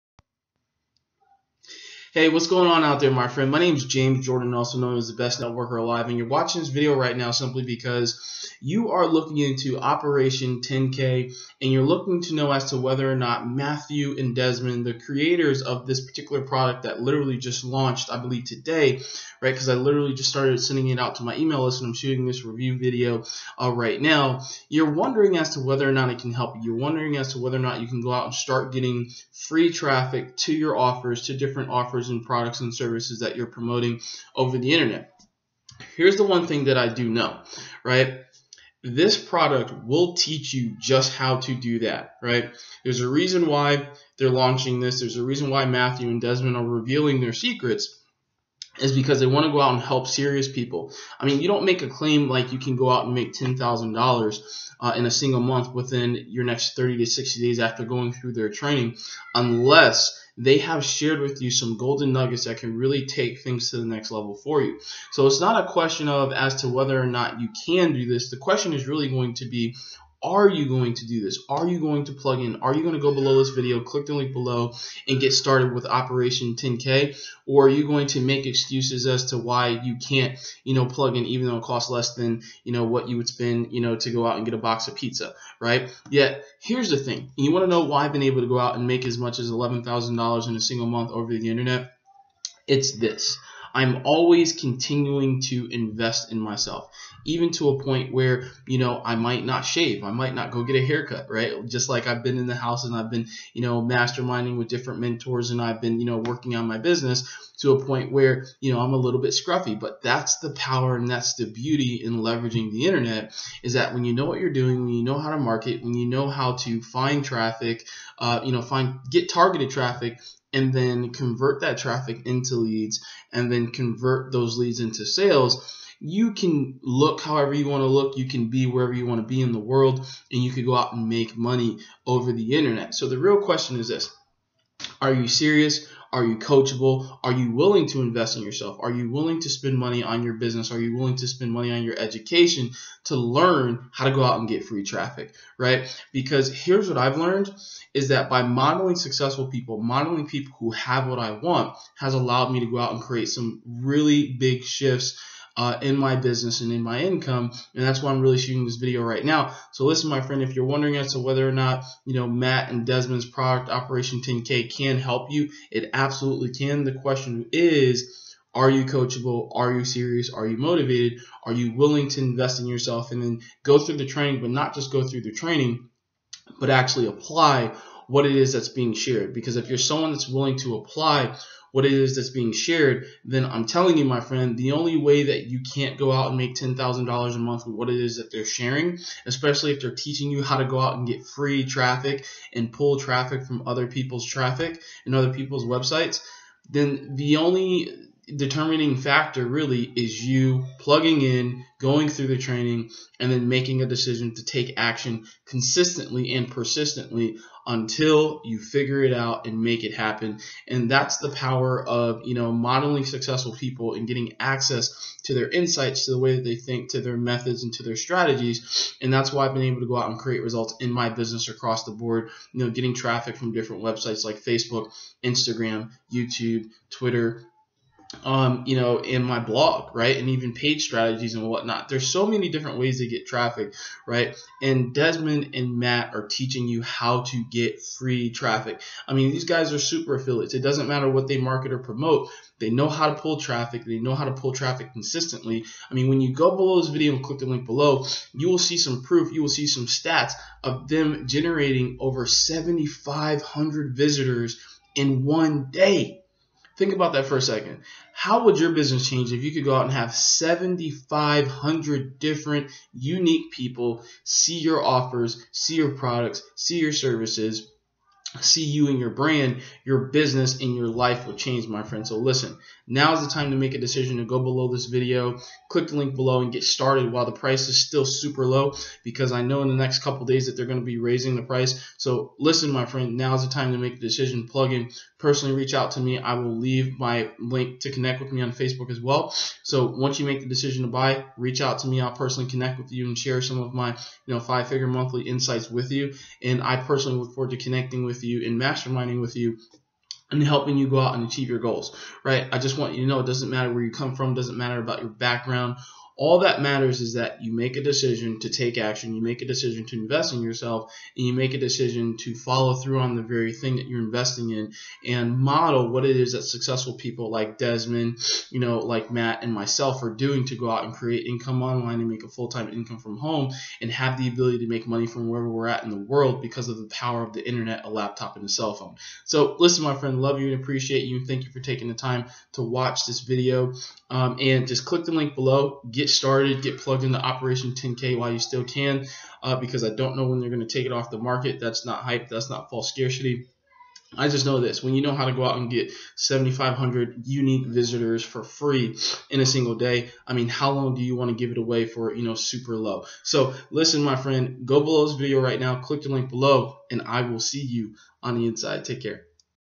Operation 10k Review Desmond Ong Matt Neer
Desmond Ong & Matt Neer just launched Operation $10k.

Considering I've personally made as much as $11k in a single month online I'm here to share with you my personal view on Operation 10k & how it can help you crush out some goals.

James Jordan

Add me as a friend on fb.